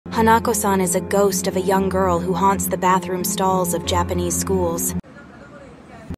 👹Hanako San:Toilet Ghost | Japanese Toilet Haunting
Introducing the Legend of Hanako San the bathroom ghost of Japan.